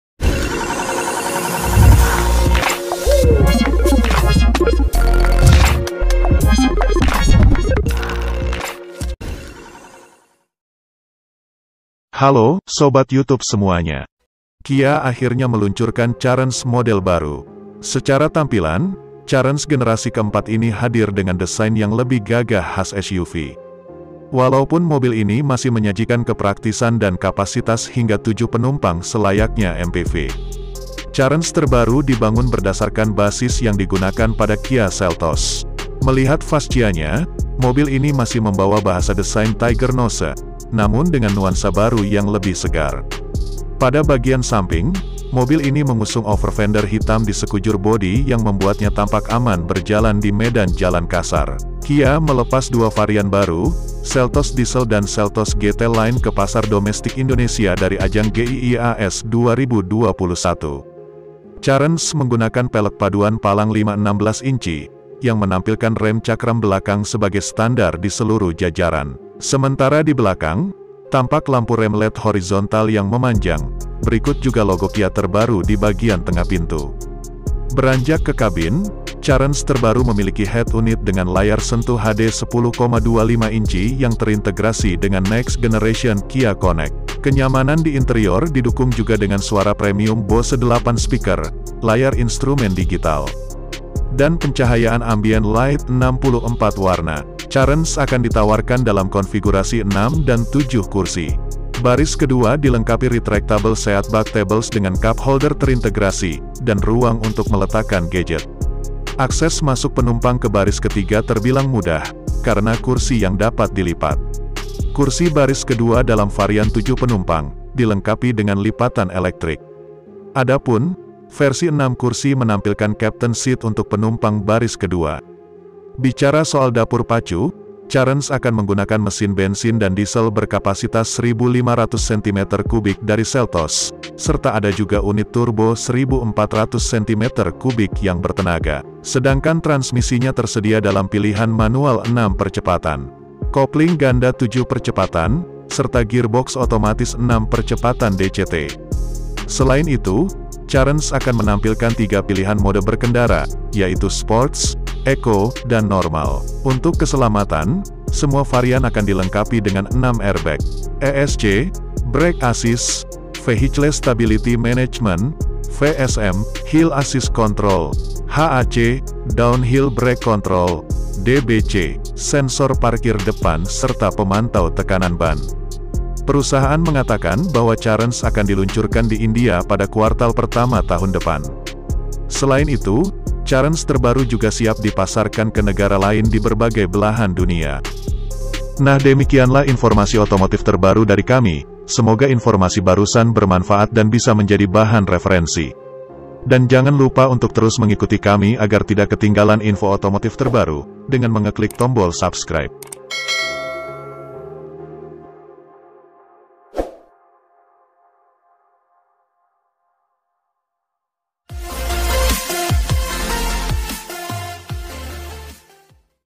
Kia Carens Generasi Baru Hadir, Tampilan Jadi Ala SUV
Carens terbaru dibangun berdasarkan basis yang digunakan pada Kia Seltos. Melihat fascia-nya, mobil ini masih membawa bahasa desain tiger nose, namun dengan nuansa baru yang lebih segar.
Pada bagian samping, mobil ini mengusung overfender hitam di sekujur bodi yang membuatnya tampak aman berjalan di medan jalan kasar.

✔️happy watching, and thank you for stopping by this channel🤝🤗

Selamat menonton 👇👇👇🤗🤝